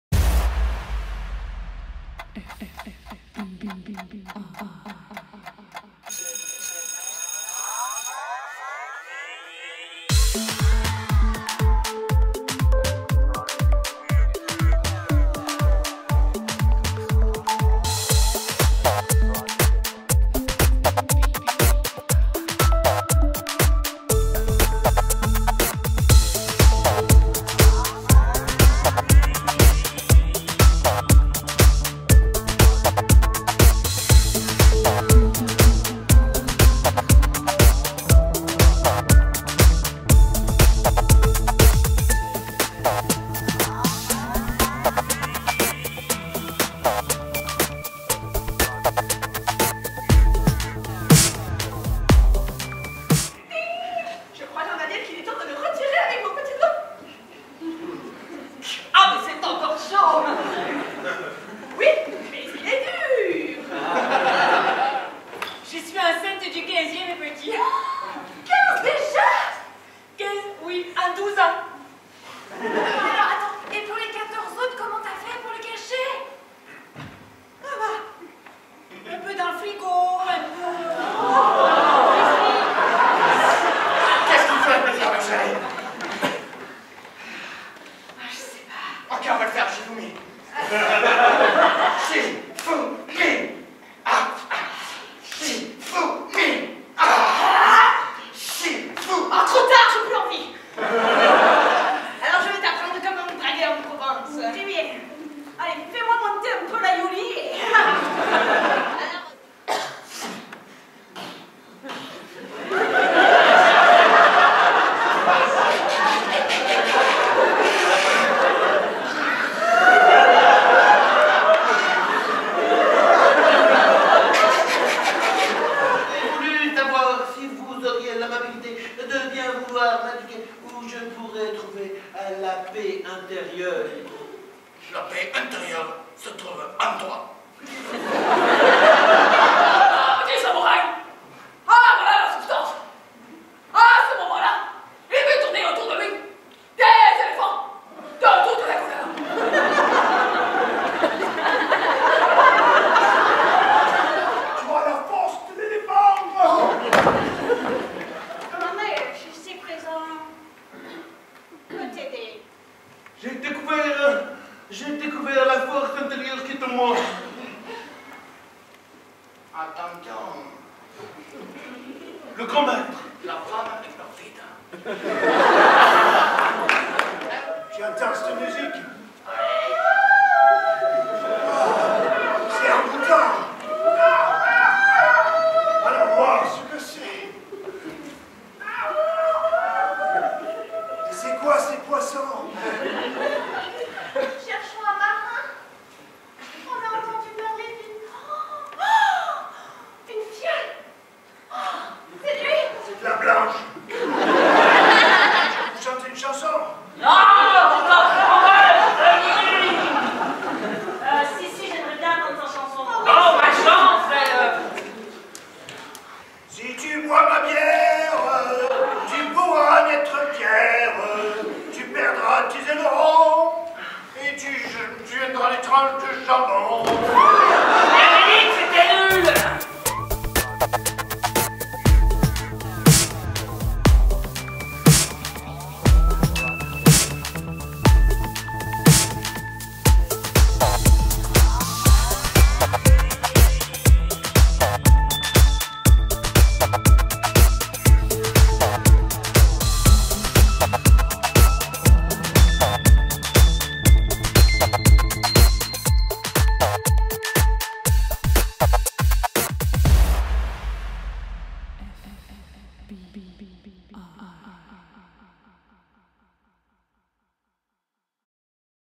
match d'impro tics Chambéry vs fbi Béziers 12 décembre 2015
Le 12 décembre l'équipe du TICS impro de Chambéry a reçu la FBI pour un match d'improvisation. La rencontre a eu lieu à la MJC du Totem de Chambéry. Le public venu nombreux a pu apprécier le talent complémentaire de chaque équipe.
A très vite en 2016 pour de nouvelles aventures :)